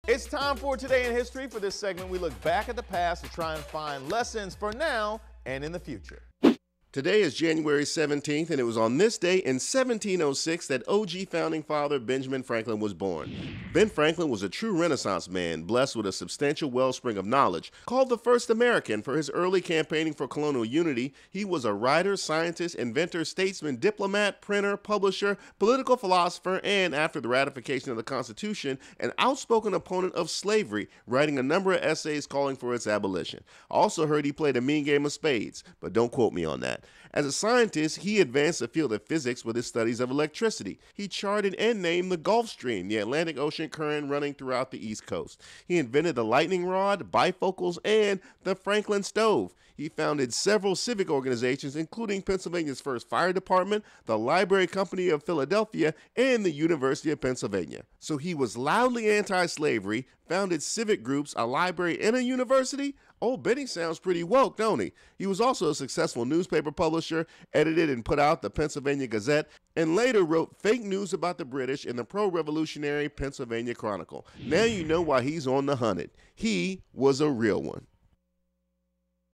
Ben Franklin was born | Today in History
One of America's forefathers had some very modern beliefs, hundreds of years ago.